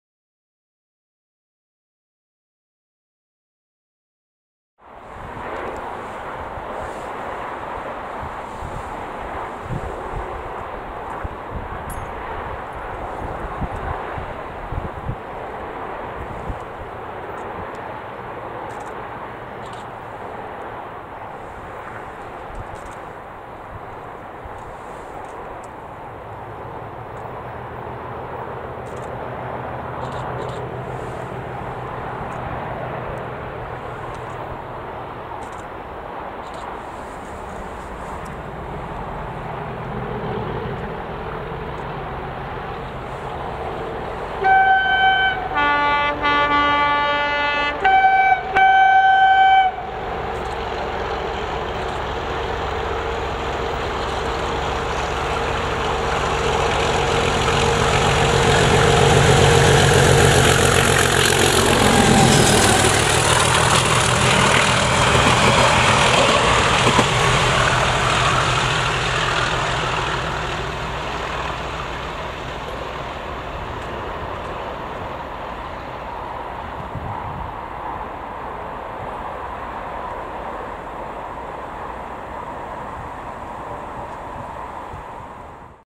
ROG 37608 thrashes past Spinks Lane dragging 755414 with tones 03/11/2020
Here we see ROG Europheonix 37608 'Andromeda' thrashing past Spinks Lane whilst dragging dead Stadler 755414 with a few tones on the 3rd of November 2020. Enjoy !